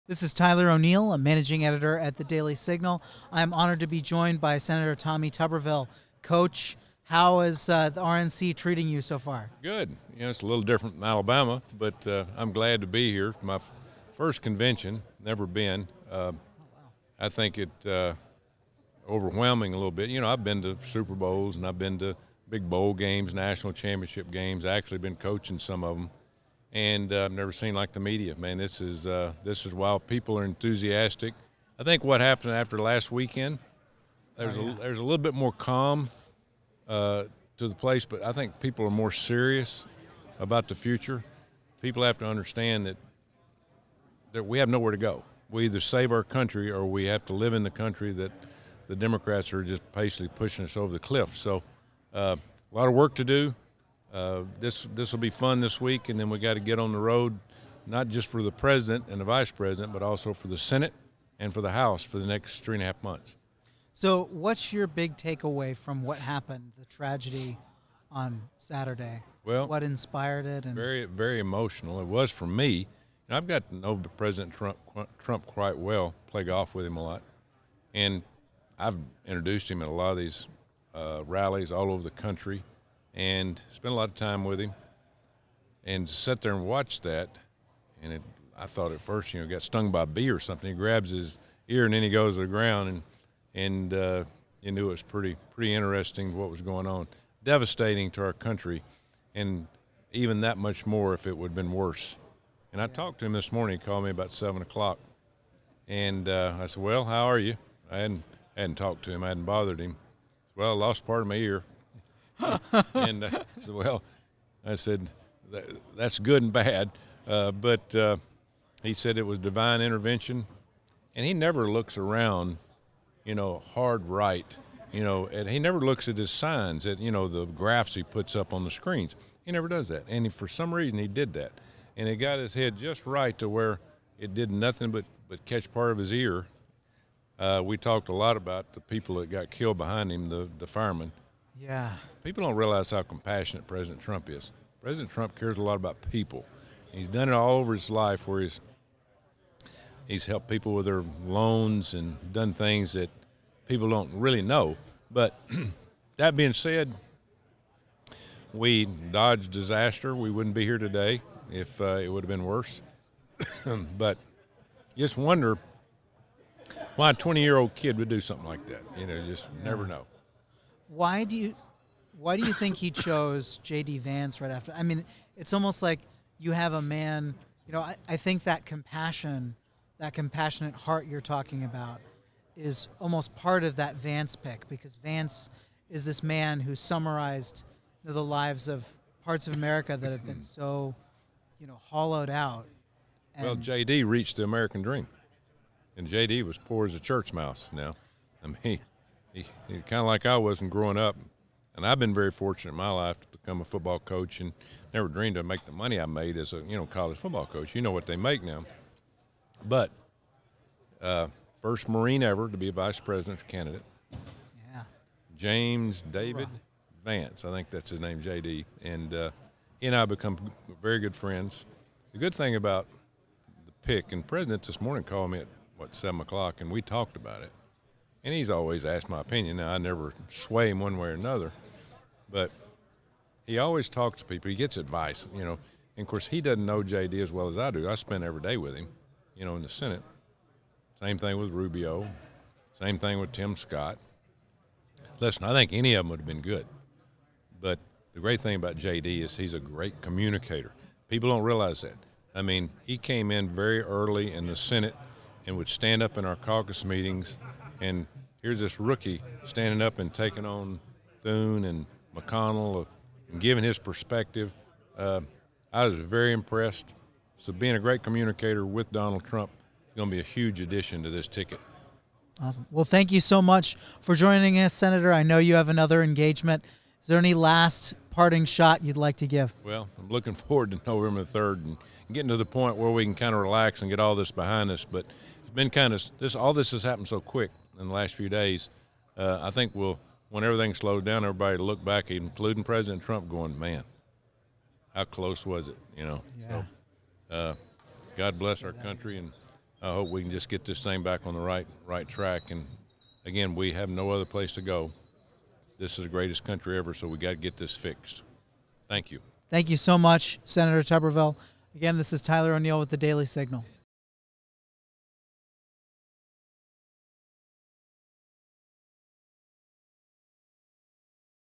‘DIVINE INTERVENTION’: Tuberville Opens Up About Call With Trump 48 Hours after Shooting
Sen. Tuberville sits down with Tyler O'Neil at the RNC in Milwuakee, Wisconsin.